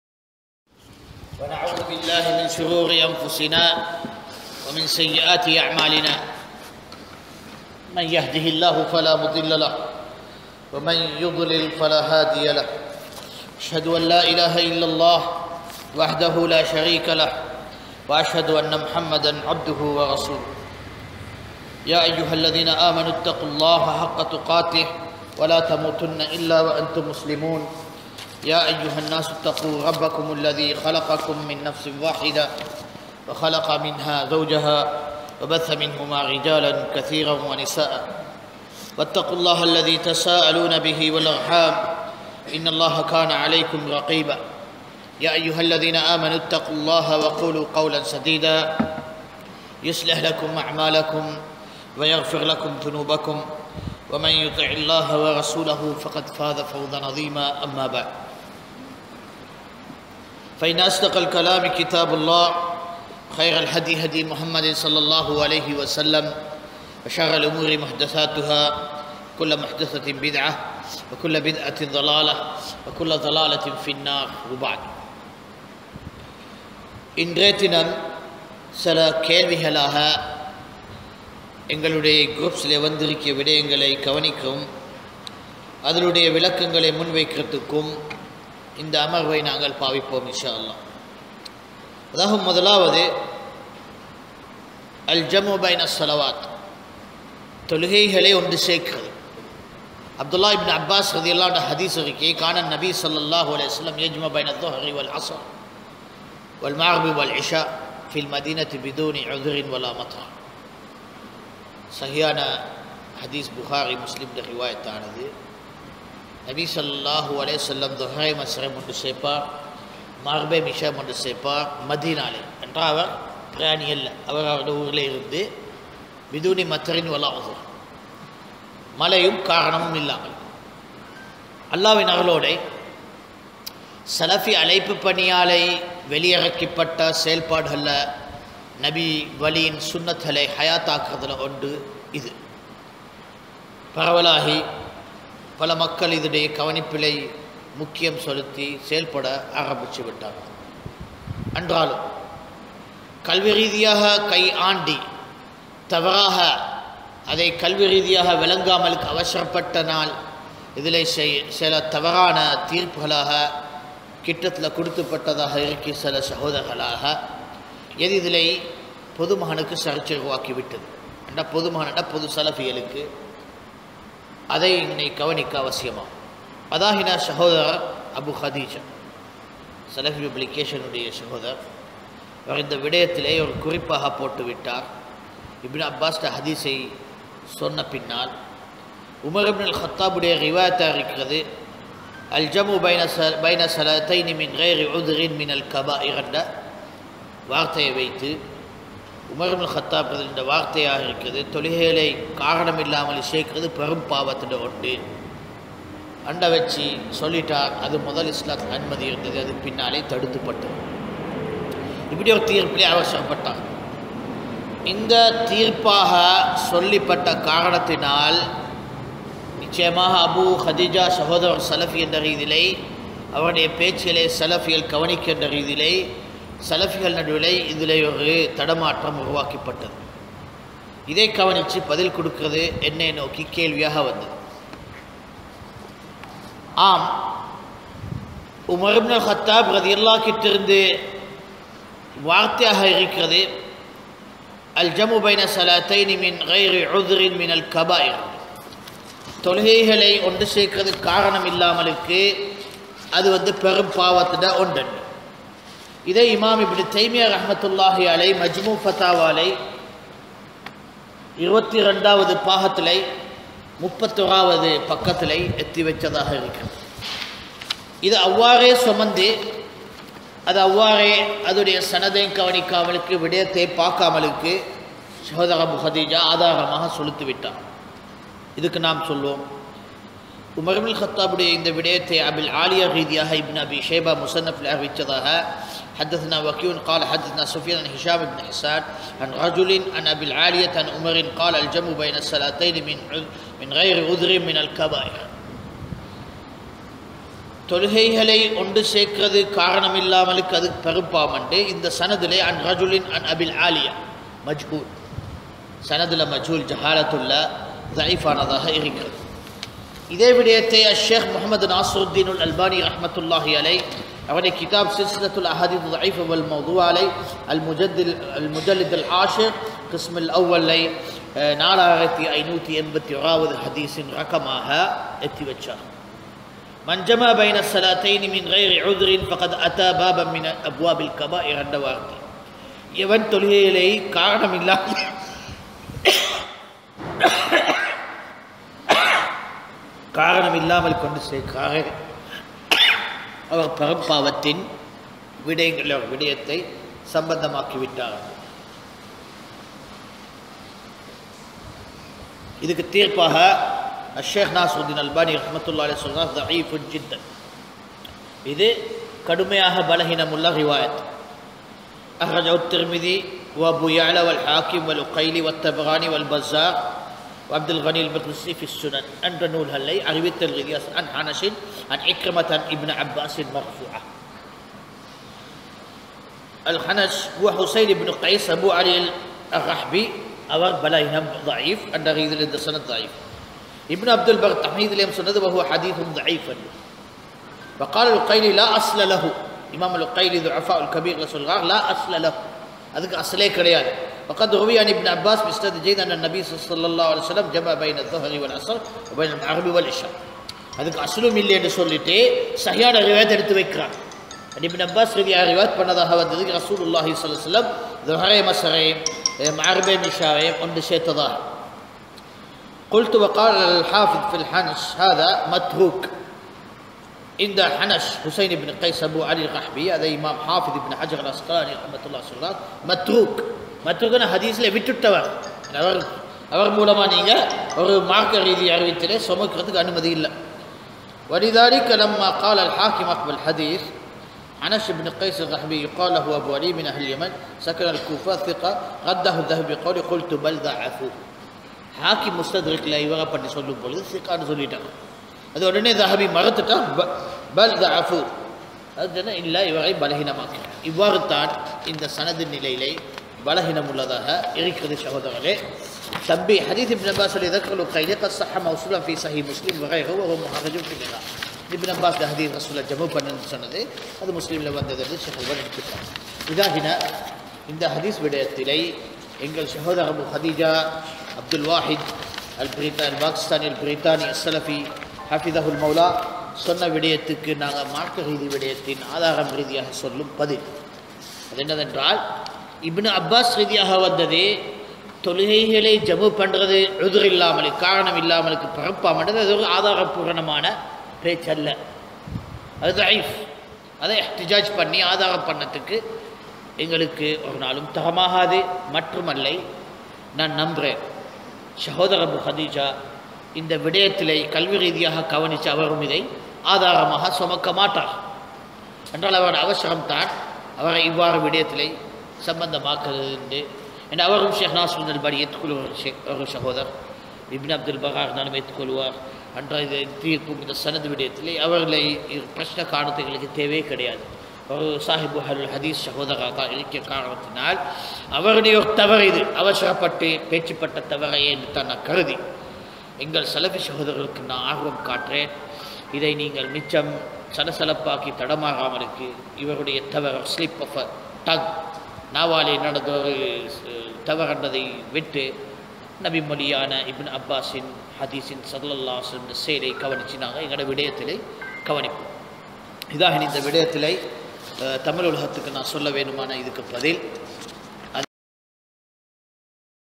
தொழுகையை காரணம் இல்லாமல் சேர்ப்பது பெரும் பாவம் என்ற அறிவிப்பின் வலுவற்ற தன்மையும் ....
சகோதரர் அபூ கதீஜாவிற்கு உபதேசமும்

உரை: ஷெய்க் யஹ்யா சில்மி அஸ் ஸைலானி ஹபிழஹுல்லாஹ்